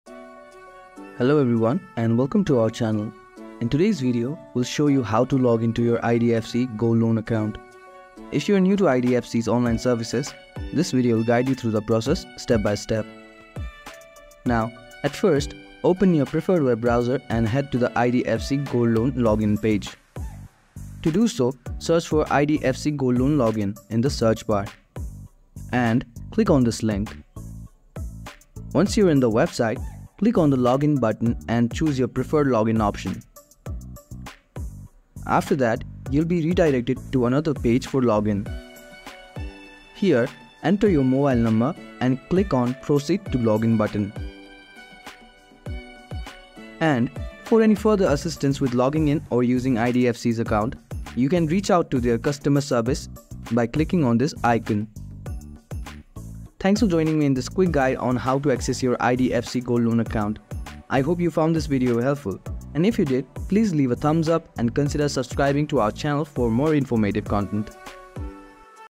How to Login IDFC Gold Loan Account 2024?
In this video, we’ll walk you through the simple steps to log in to your IDFC Gold Loan account in 2024. We’ll cover everything from navigating to the official IDFC Bank website, locating the Gold Loan login section, and entering your User ID and Password correctly, to registering as a new user and troubleshooting common issues. Whether you’re checking your loan balance, reviewing your repayment schedule, or managing your account details, this guide will help you log in quickly and securely. Have questions or feedback?